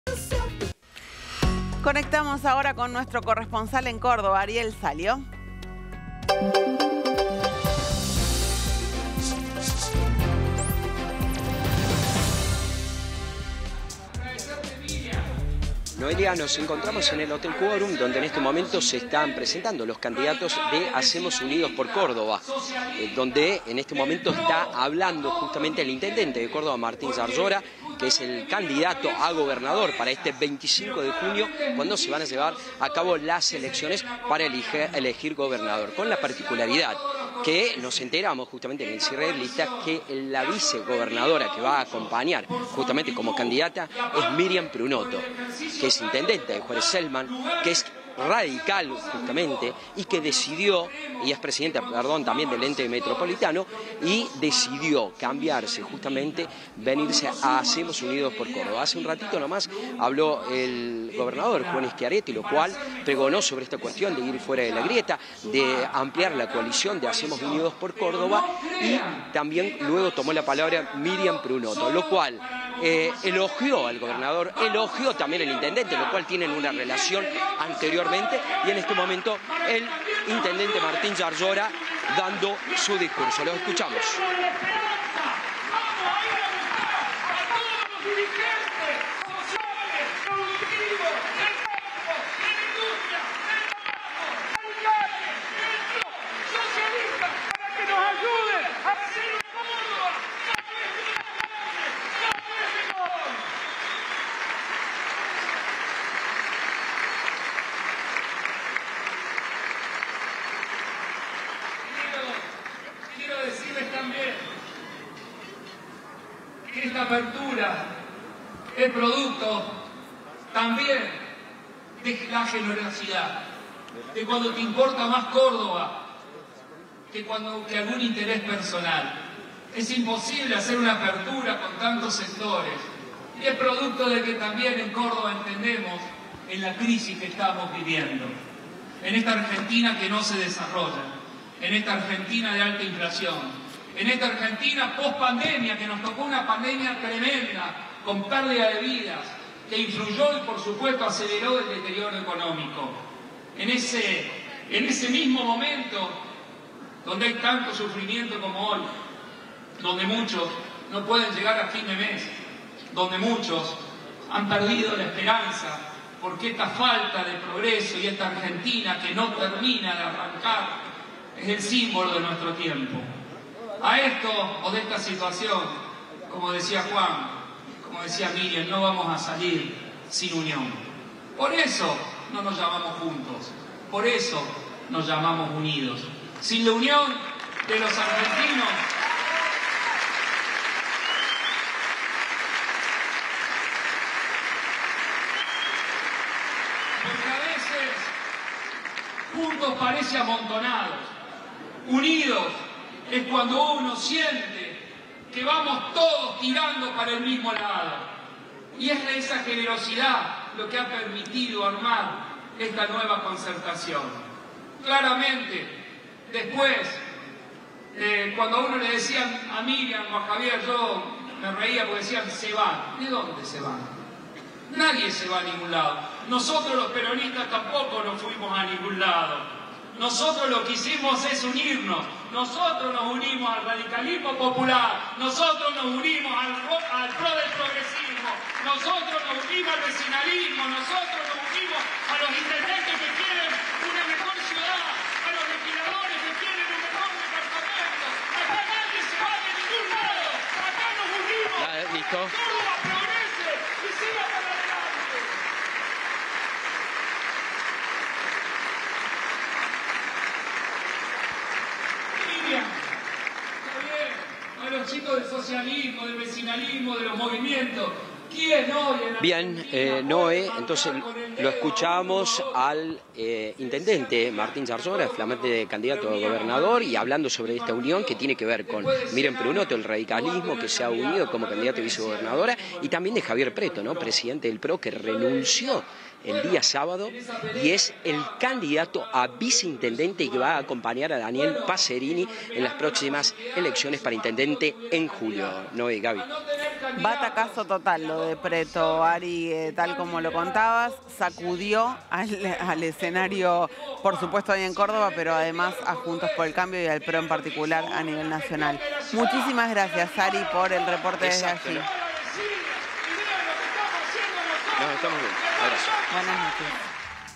🔹 Hacemos Unidos por Córdoba lanzó su campaña: "Hoy damos el primer paso para construir el triunfo"
La coalición oficialista presentó a sus candidatos en un acto en el Hotel Quórum de la capital provincial. Juan Schiaretti, precandidato presidencial, abrió el evento para que la fórmula para la gobernación, integrada por Martín Llaryora y Myrian Prunotto, protagonice el encuentro.

La corresponsalía de IP Noticias nos trae toda la información desde el lugar de los hechos.